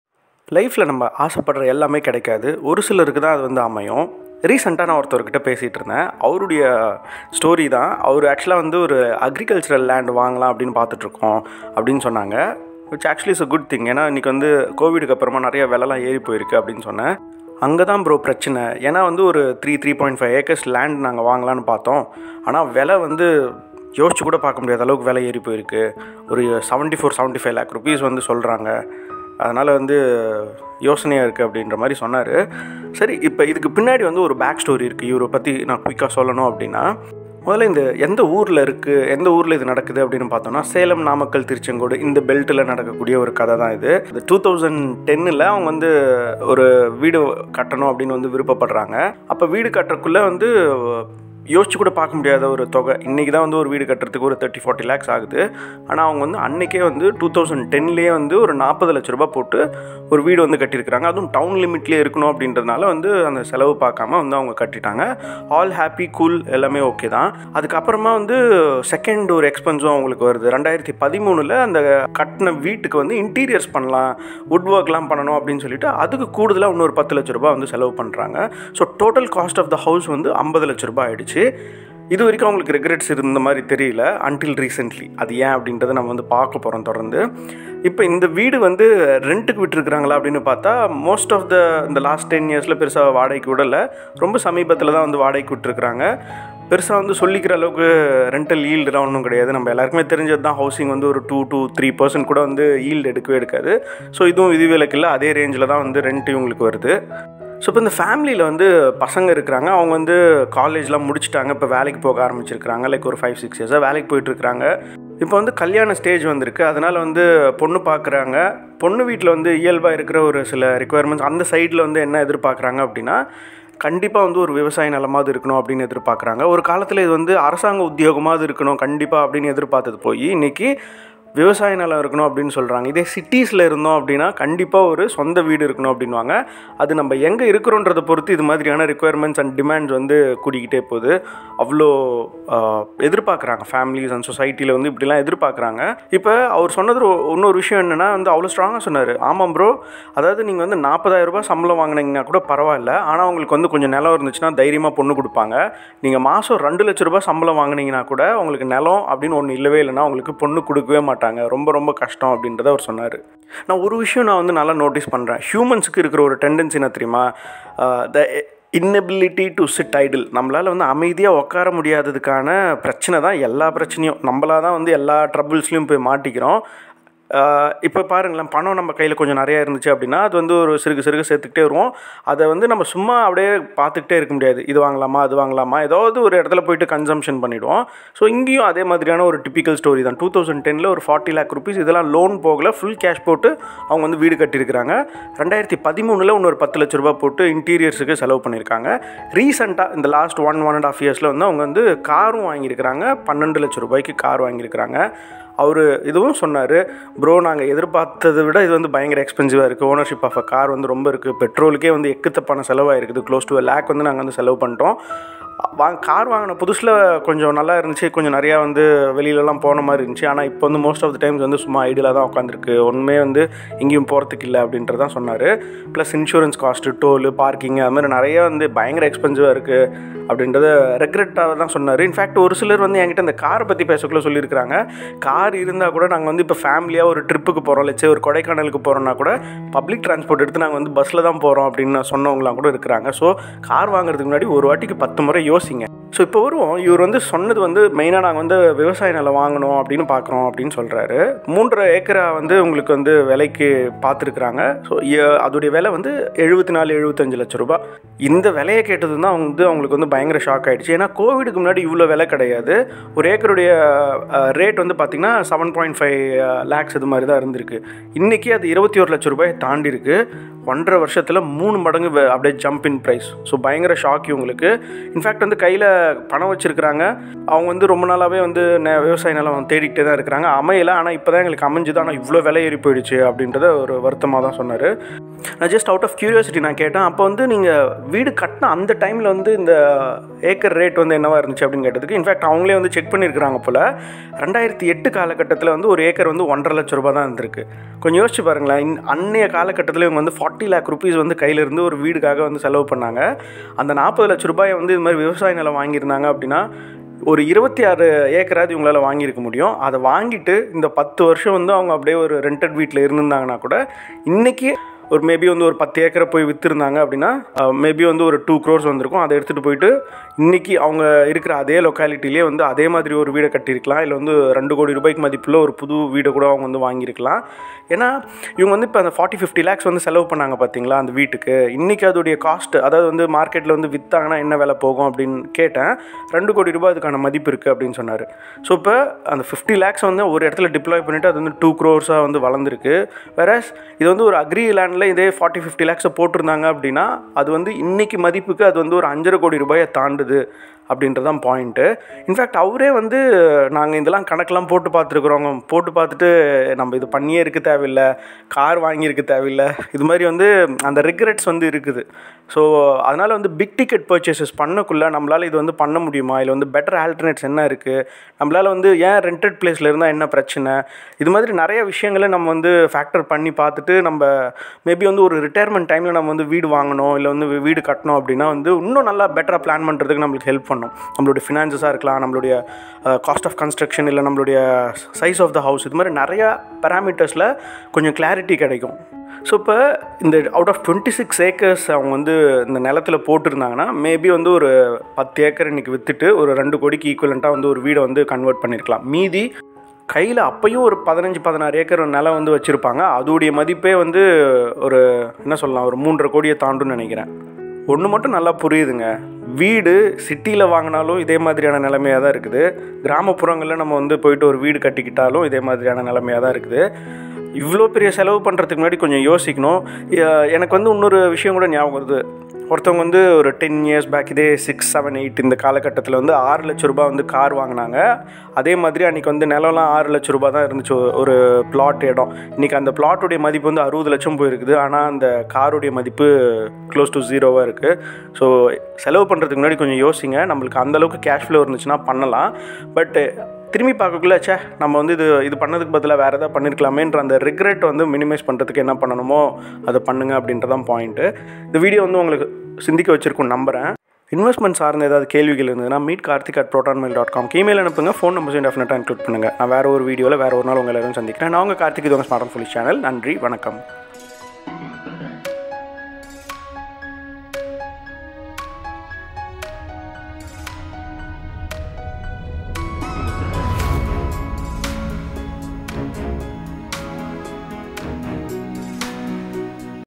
Family Finances: 50 லட்சம் வீட்டுக்கு செலவு செய்தோம் | இன்று வருத்தமா இருக்கு
We spent 50 lakhs for a house in 2010, today we regret the decision.